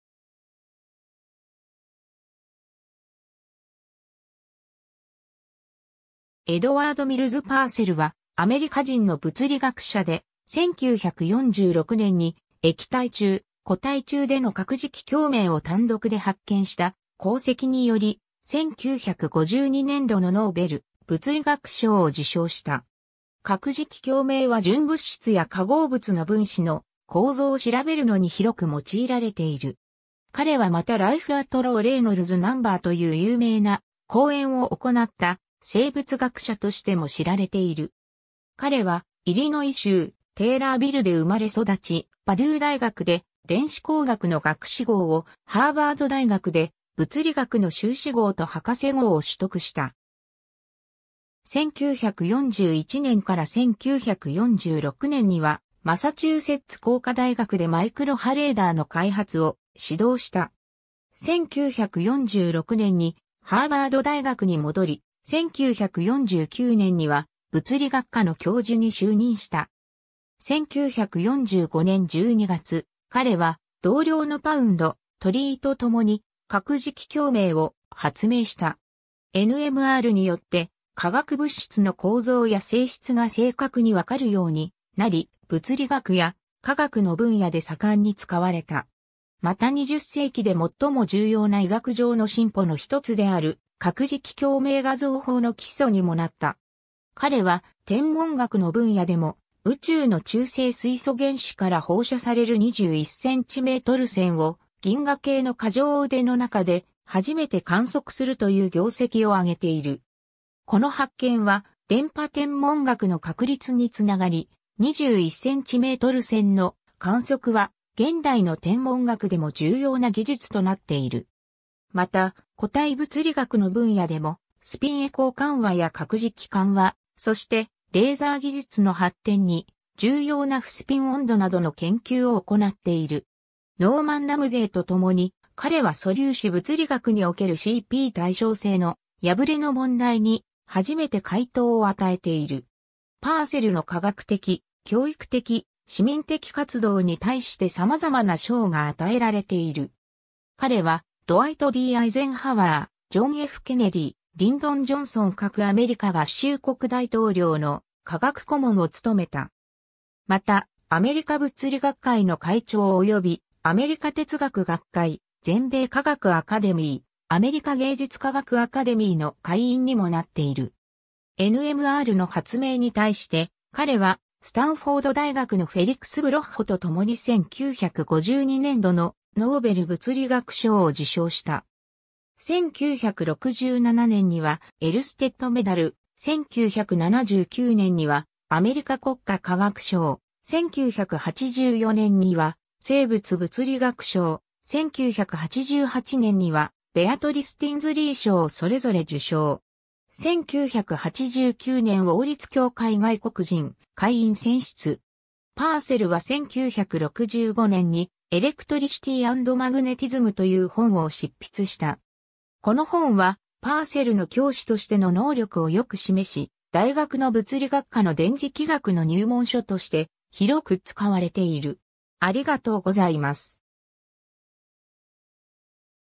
エドワード・ミルズ・パーセル
エドワード・ミルズ・パーセル（Edward Mills Purcell, 1912年8月30日 – 1997年3月7日）は、アメリカ人の物理学者で、1946年に液体中、固体中での核磁気共鳴を単独で発見した功績により、1952年度のノーベル物理学賞を受賞した。
核磁気共鳴は純物質や化合物の分子の構造を調べるのに広く用いられている。
彼はまたLife at Low Reynolds Number（低レイノルズ数における生命）という有名な講演を行った生物学者としても知られている。
 彼はイリノイ州テーラービルで生まれ育ち、パデュー大学で電子工学の学士号を、ハーバード大学で物理学の修士号と博士号を取得した。
1941年から1946年にはマサチューセッツ工科大学でマイクロ波レーダーの開発を指導した。
1946年にハーバード大学に戻り、1949年には物理学科の教授に就任した。
 1945年12月、彼は同僚のパウンド、トリーとともに核磁気共鳴（NMR）を発明した。
NMRによって化学物質の構造や性質が正確に分かるようになり、物理学や化学の分野で盛んに使われた。
また20世紀で最も重要な医学上の進歩の1つである核磁気共鳴画像法の基礎にもなった。
 彼は天文学の分野でも、宇宙の中性水素原子から放射される21cm線を銀河系の渦状腕の中で初めて観測するという業績を上げている。
この発見は電波天文学の確立につながり、21cm線の観測は現代の天文学でも重要な技術となっている。
また固体物理学の分野でも、スピンエコー緩和や核磁気緩和、そしてレーザー技術の発展に重要な負スピン温度などの研究を行っている。
ノーマン・ラムゼーとともに、彼は素粒子物理学におけるCP対称性の破れの問題に初めて解答を与えている。
 パーセルの科学的、教育的、市民的活動に対して様々な賞が与えられている。
彼はドワイト・D・アイゼンハワー、ジョン・F・ケネディ、リンドン・ジョンソン各アメリカ合衆国大統領の科学顧問を務めた。
またアメリカ物理学会の会長およびアメリカ哲学学会、全米科学アカデミー、アメリカ芸術科学アカデミーの会員にもなっている。
NMRの発明に対して、彼はスタンフォード大学のフェリクス・ブロッホとともに1952年度のノーベル物理学賞を受賞した。
1967年にはエルステッド・メダル、1979年にはアメリカ国家科学賞、 1984年には生物物理学賞、1988年にはベアトリス・ティンズリー賞をそれぞれ受賞。
1989年王立協会外国人会員選出。
 パーセルは1965年にElectricity and Magnetismという本を執筆した。
この本はパーセルの教師としての能力をよく示し、大学の物理学科の電磁気学の入門書として広く使われている。